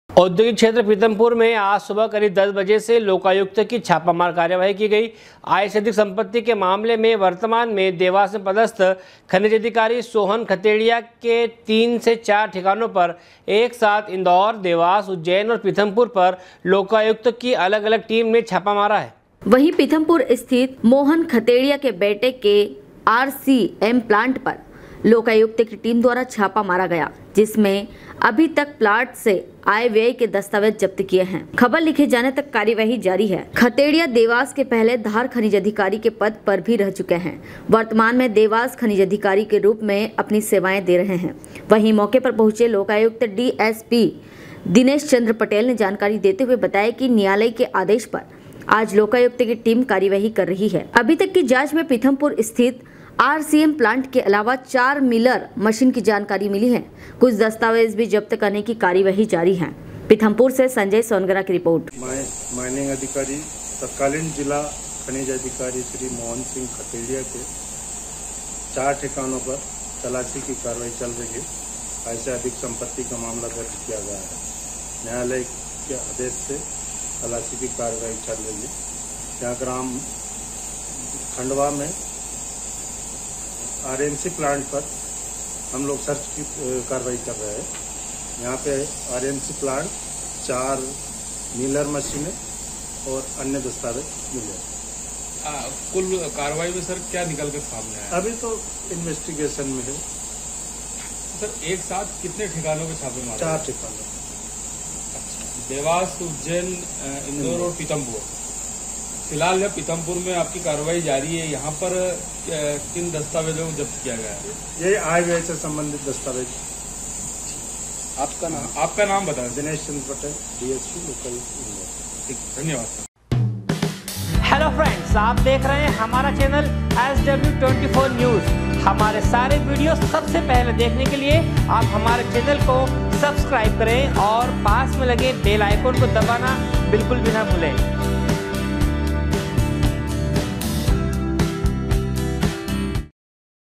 औद्योगिक क्षेत्र पीथमपुर में आज सुबह करीब 10 बजे से लोकायुक्त की छापा मार कार्यवाही की गई।
औद्योगिक क्षेत्र पीथमपुर में आज सुबह करीब 10 बजे से लोकायुक्त की छापा मार कार्यवाही की गई। आय से अधीक संपत्ति के मामले में वर्तमान में देवास में पदस्थ खनिज अधिकारी सोहन खतेड़िया के तीन से चार ठिकानों पर एक साथ इन्दौर ,देवास, उज्जैन, और पीथमपुर पर लोकायुक्त की अलग अलग टीम ने छापा मारा।
पीथमपुर से संजय सोनगरा की रिपोर्ट ।